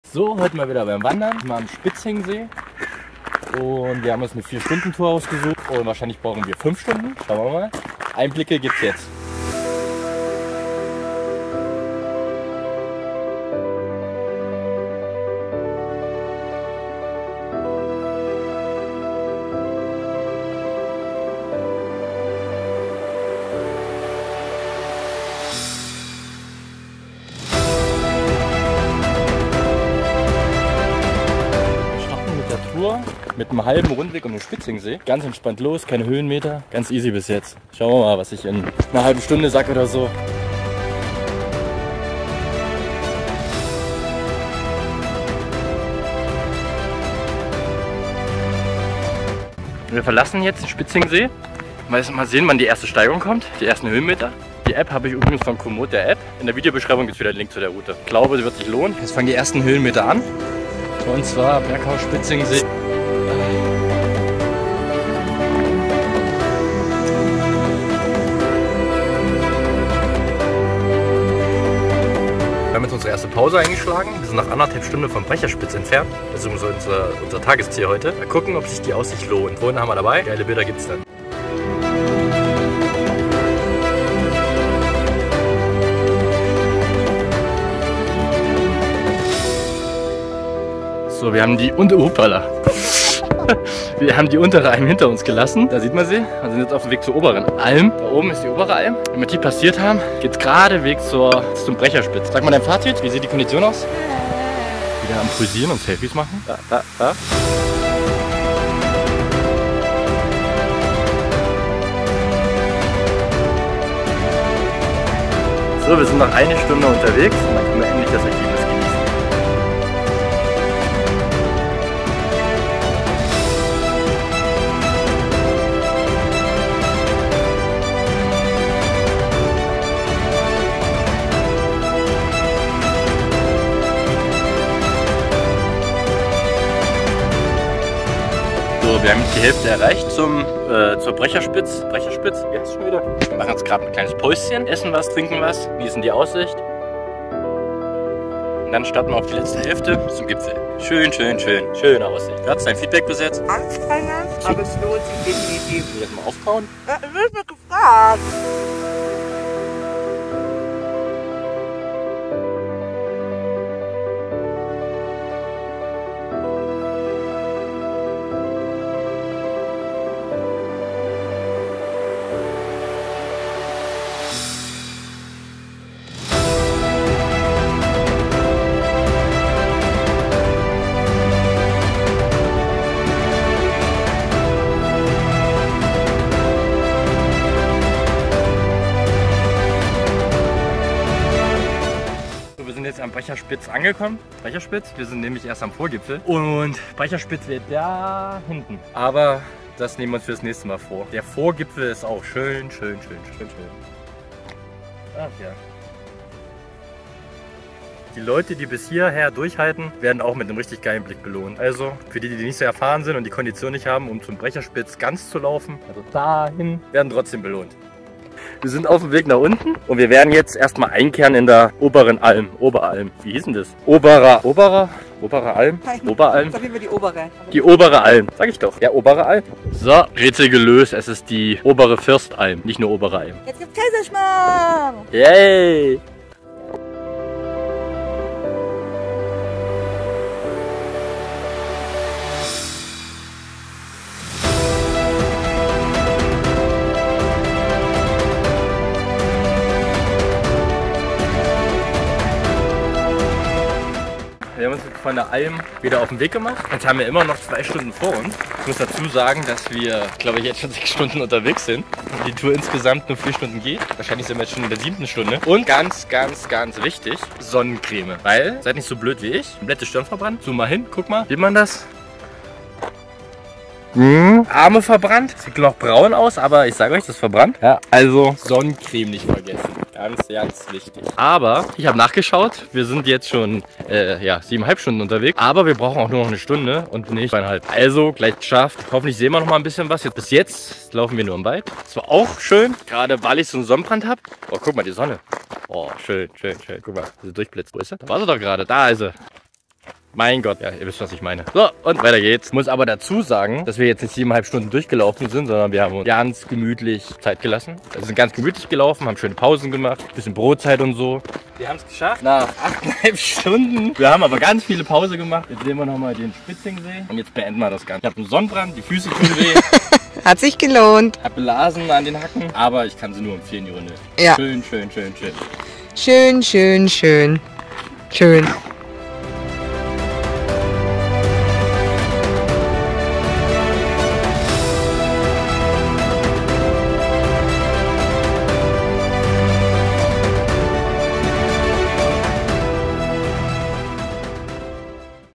Wandern am Spitzingsee: Brecherspitz Vorgipfel & Oberen Firstalm | Bayern | 12 km Route
In diesem Video zeige ich euch eine schöne Wanderung um den Spitzingsee in den Bayerischen Alpen. Die Route ist etwa 12 Kilometer lang und führt über idyllische Seewiesen, vorbei am Brecherspitz Vorgipfel und zur Oberen Firstalm.

Highlights:
Idyllischer Seeblick
Schöne Almwiesen
Brecherspitz Vorgipfel
Obere Firstalm
Traumausblick auf die Obere Firstalm

Toureninfos:
Startpunkt: Parkplatz Spitzingsee
Streckenlänge: ca. 12 km
Gehzeit: ca. 5 Stunden
Schwierigkeitsgrad: mittel

Route:
Die Route kann auf der Komoot-App heruntergeladen werden.

Wenn ihr Lust auf eine schöne Wanderung in den Bayerischen Alpen habt, dann schaut euch das Video an und ladet euch die Route herunter.

Einleitung: 0:00
Tourdaten: 0:09
Drohnenaufnahmen 1: 0:27
Tourstart: 0:31
Drohnenaufnahmen 2: 0:40
Ende Spitzingsee: 0:49
Pause 1: 1:12
Aufstieg Untere Alm: 1:20
Aufstieg Obere Alm: 1:29
Aufstieg Brecherspitz Vorgipfel: 1:55
Pause 2: 2:19
Drohne Brecherspitz Vorgipfel: 2:38
Abbruch Brecherspitz Vorgipfel: 3:13
Wie heißt die Alm?: 3:38
Weg zur Oberen Alm: 3:56
Abstieg: 4:14
Endfazit: 5:11